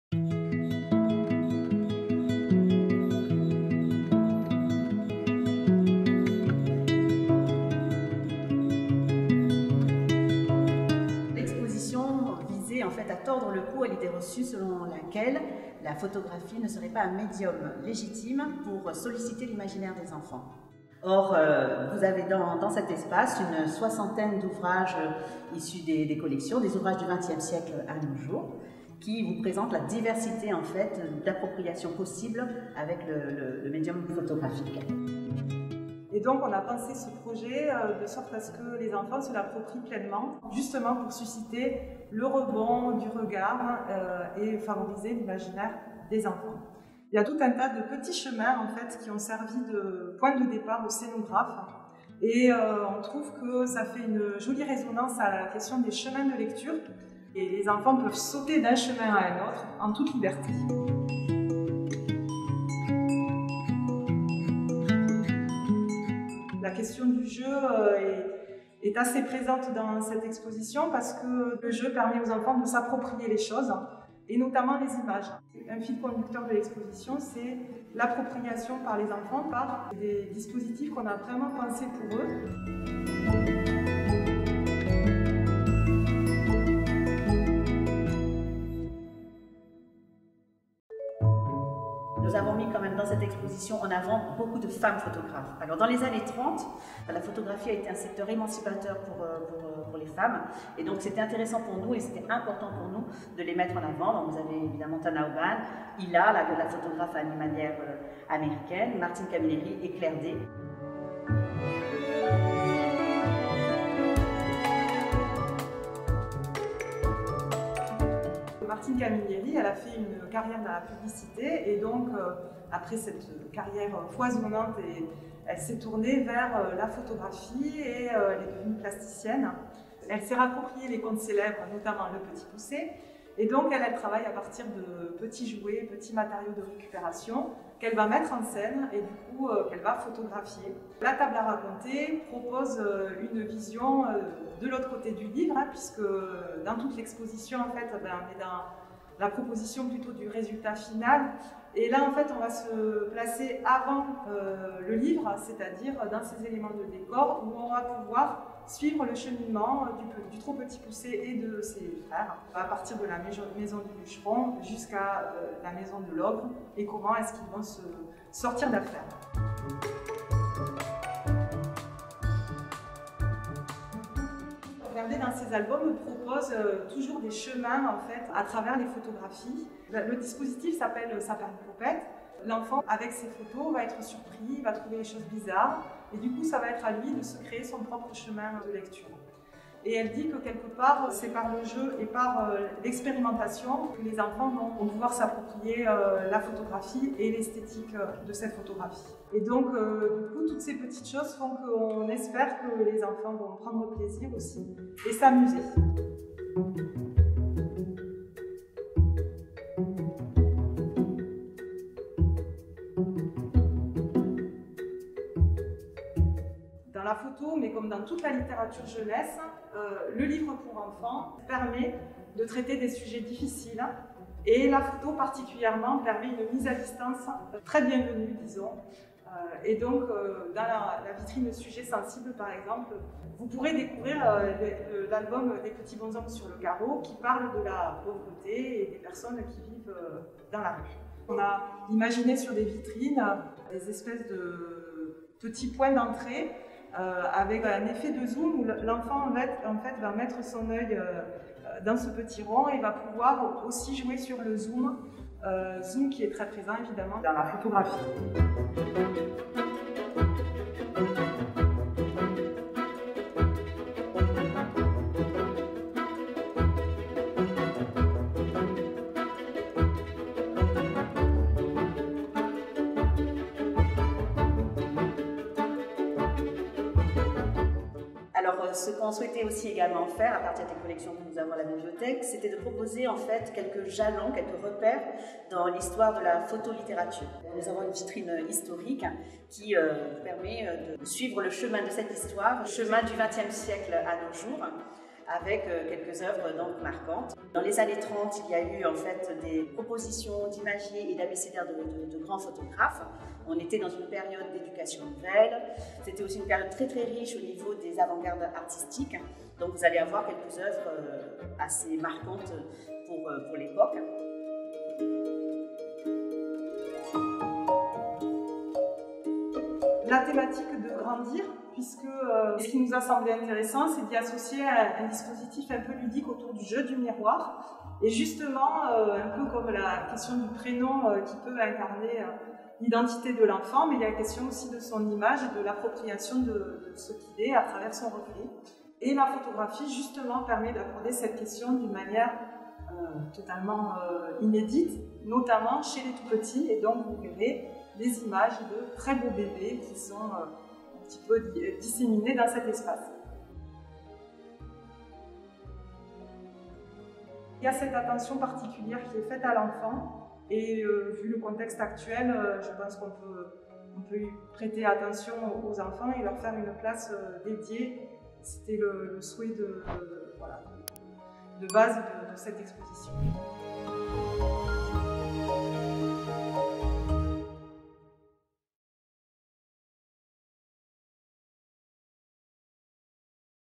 Visite virtuelle de l’exposition Clic Clac
exposition Clic Clac, la photographie dans la littérature jeunesse présentée à la Médiathèque José Cabanis jusqu’au 20 septembre propose une ardente illustration de la richesse de la photographie dans la littérature jeunesse.